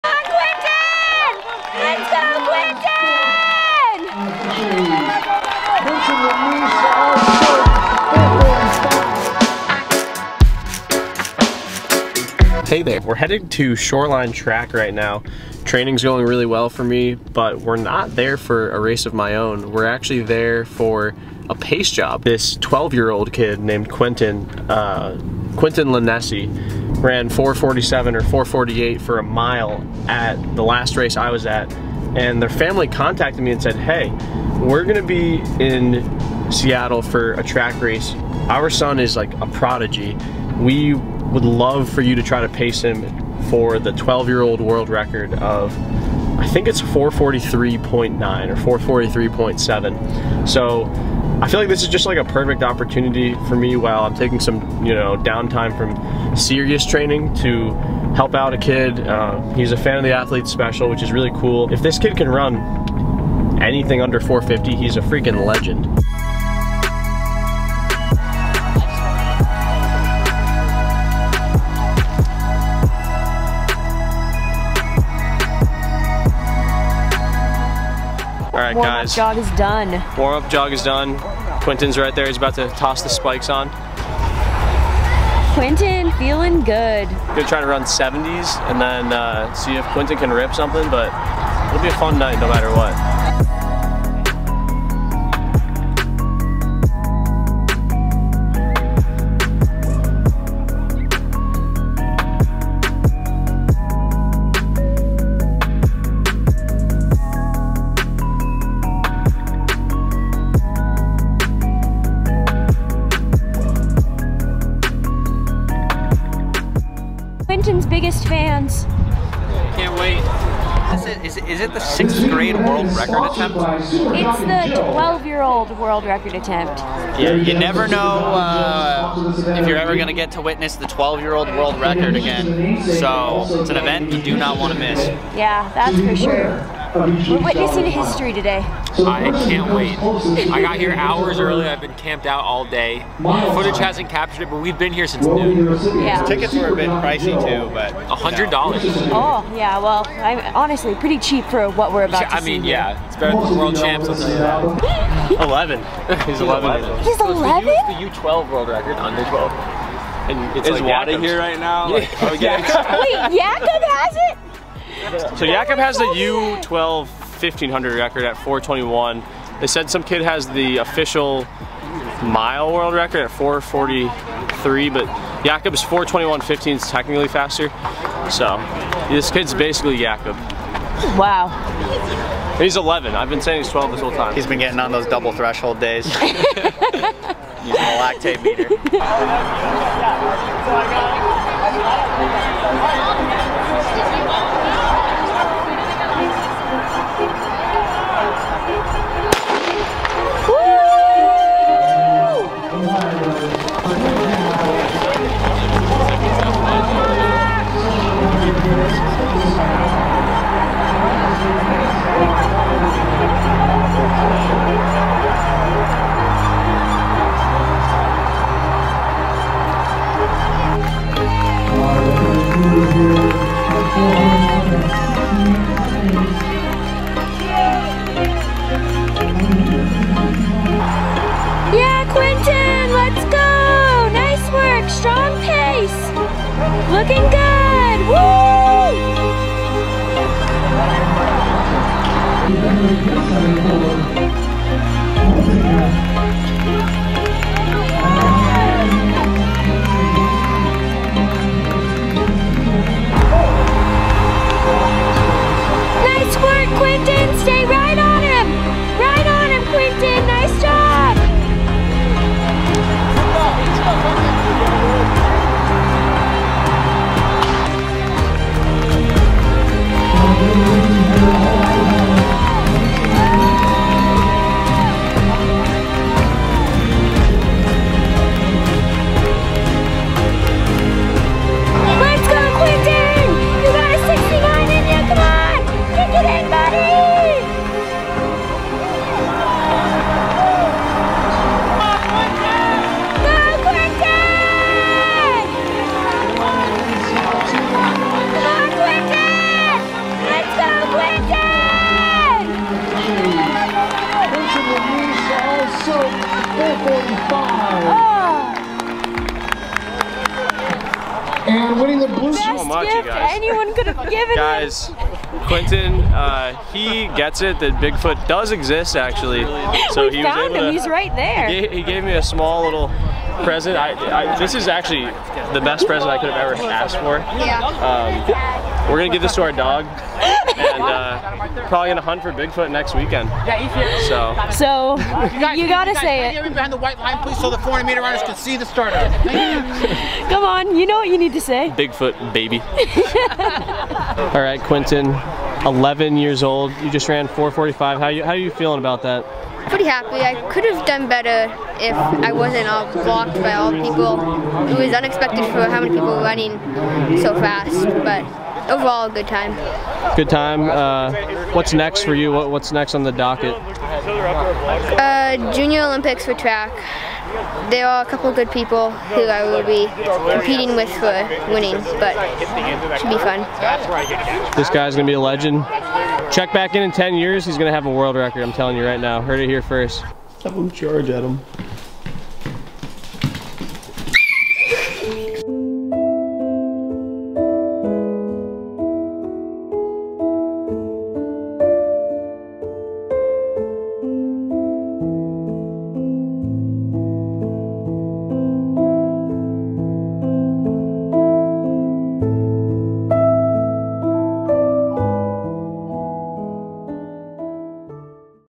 11 Year Old Runner Attempts Mile Time World Record
11 year old runner Quenton Lanese attempts to break the world record for fastest mile time ran by an athlete under 12 years old. The current world record is 4 minutes 43.7 seconds. It was a pleasure for me (@TheAthleteSpecial) to run alongside this BEAST as the pacer for his track race in Seattle!